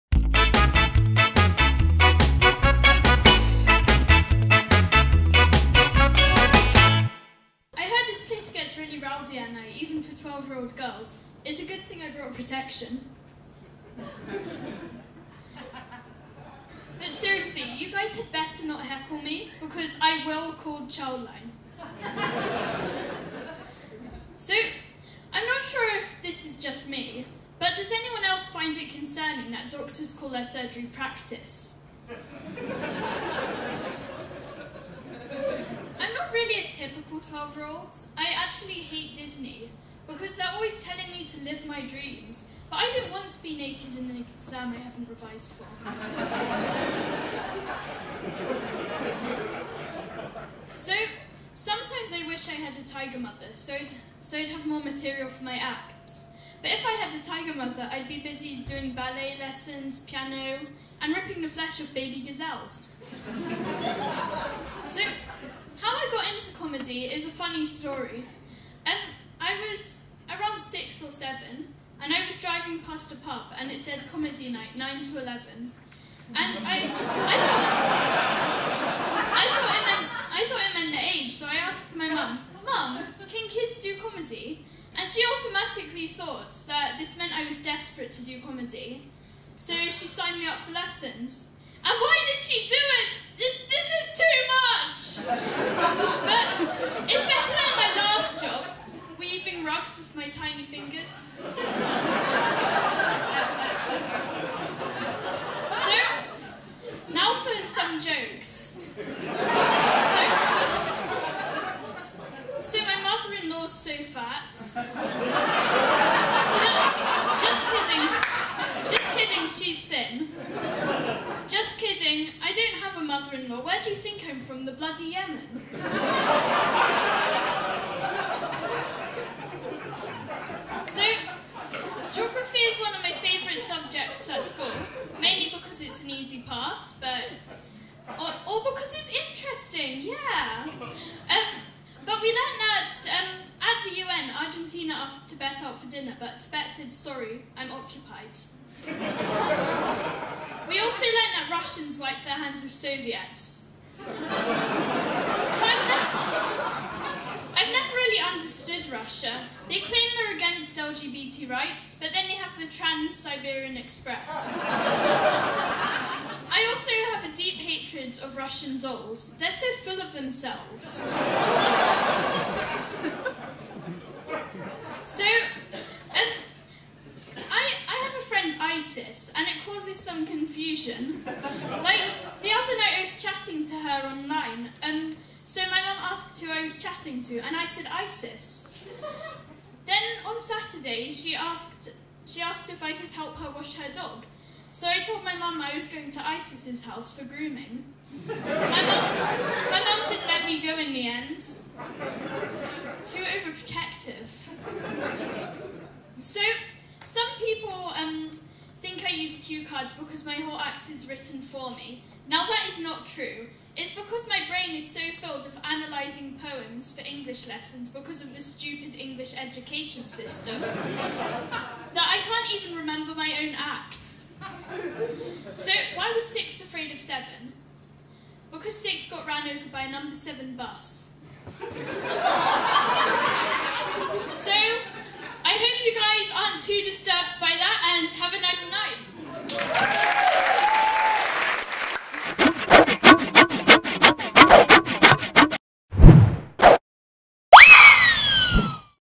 Grace The Child 2
From Chortle's Fast Fringe. Grace The Child is on at Bob's Blundabus at Potterow Undepass at 4.30pm on Saturday August 8 and Sunday August 9